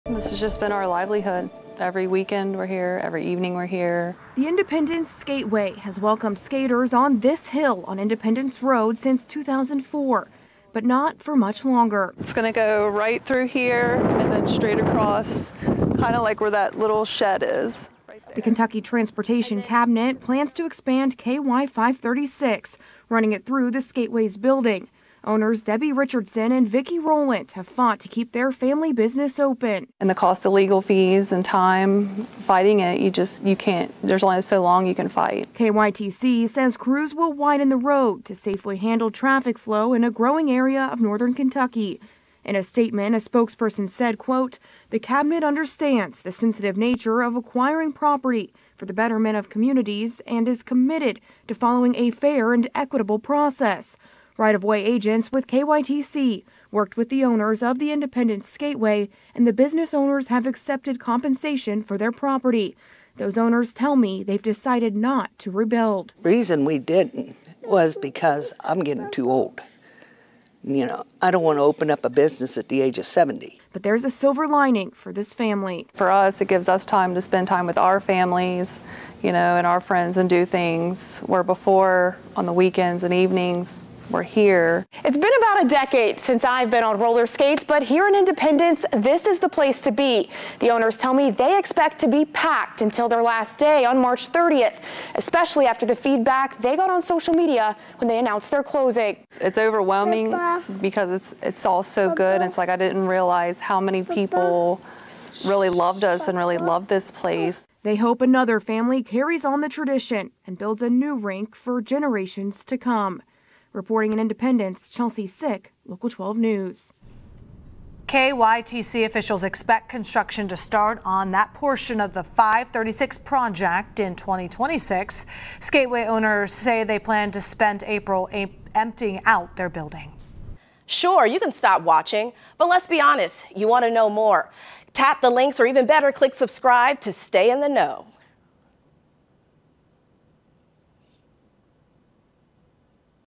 Kentucky skating rink closing after nearly 20 years
"This has just been our livelihood. Every weekend, we're here. Every evening, we're here," said co-owner Debbie Richardson